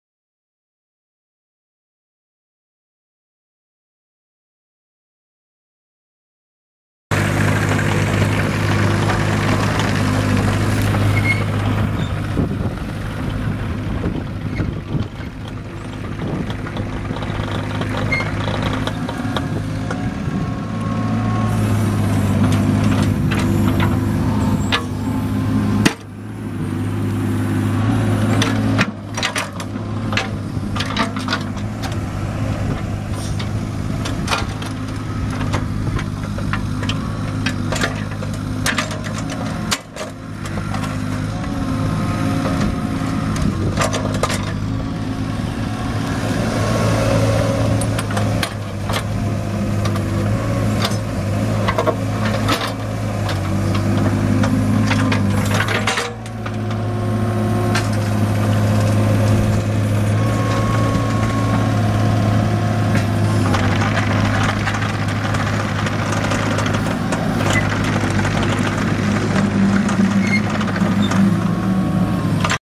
1999 IHI 30NX compact excavator for sale | sold at auction July 16, 2015
1999 IHI 30NX compact excavator
2,069 hours on meter
Isuzu three cylinder diesel engine
Hydrostatic transmission
OROPS
Drive lever controls
Joystick bucket controls
Suspension seat
Dual operating control switch
Backfill blade
16"W bucket
Rubber tracks
6,560 lbs. base operating weight
Serial WE000205
View repairs